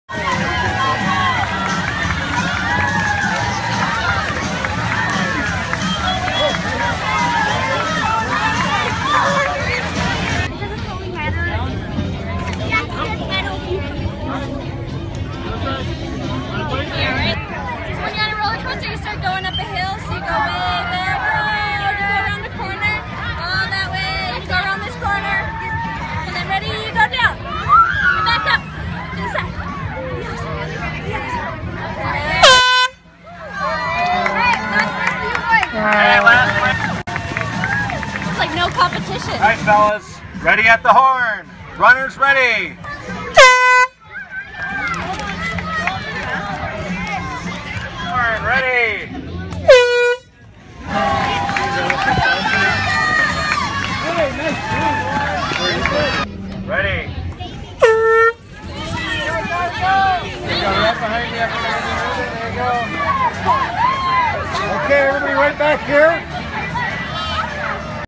Whipper Snapper Races For Kids
Kids running the Whipper Snapper Races for Kids at Bayfront Festival Park in Duluth.